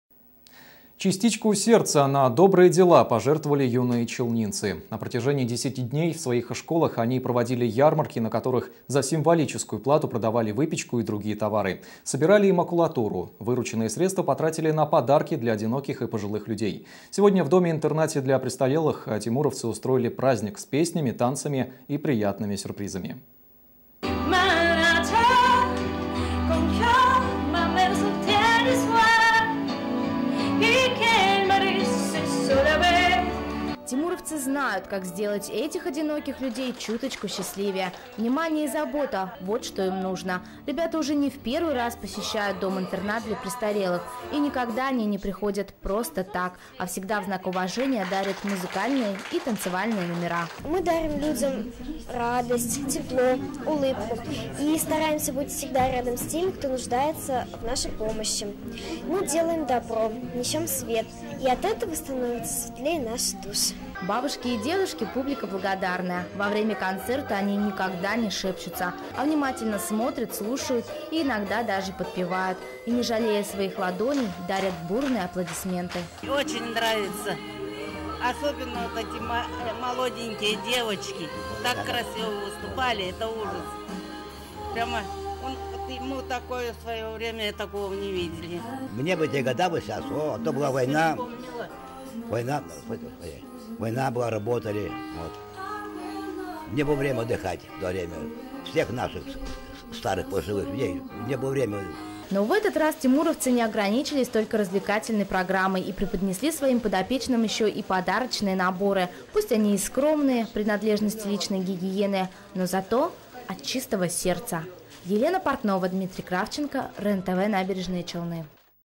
Частичку сердца -- на добрые дела
Тимуровцы устроили праздник в доме-интернате для престарелых